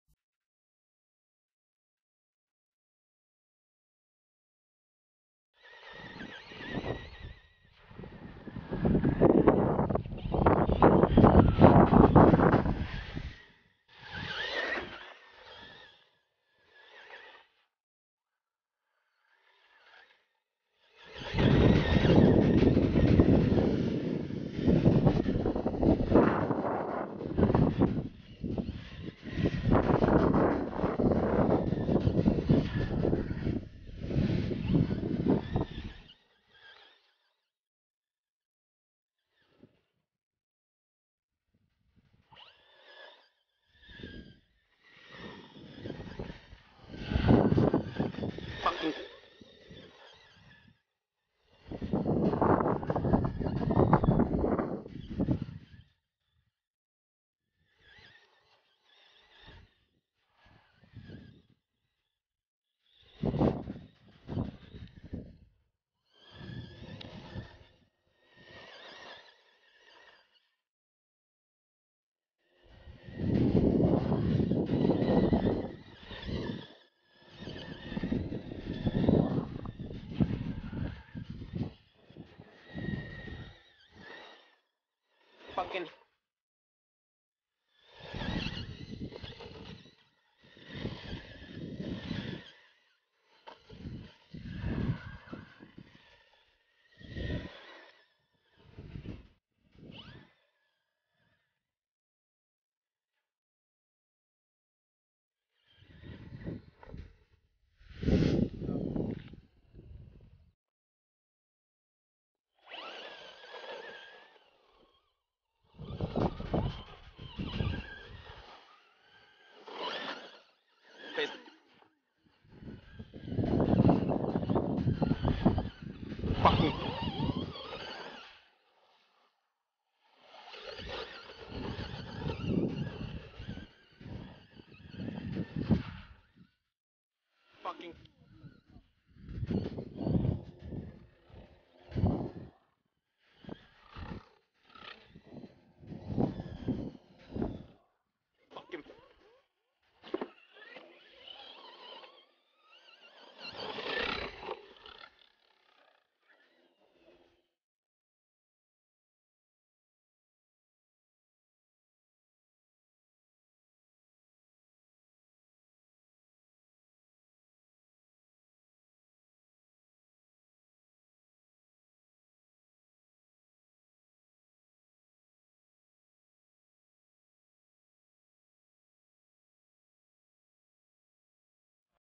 Traxxas Slash,,wet track, pioneer V's 2.8 trenchers
Traxxas Slash 2wd, wet sand/clay track..High speed bashing, pioneers skating on surface, struggle to get traction slowing car down...2.8 trenches had more traction,needs  drain holes in tyres,some ballooning..After running through muddy water,be sure to dry out and lube wheel bearings, also check drive shafts as they seize up!!